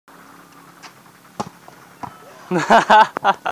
LATIHAN SMASH BOLA VOLI_DADAN SAEFULOH&RAHMAT HIDAYAT&ERIK
Latihan yang bertempat di lapangan bola voli PBV. PROVEX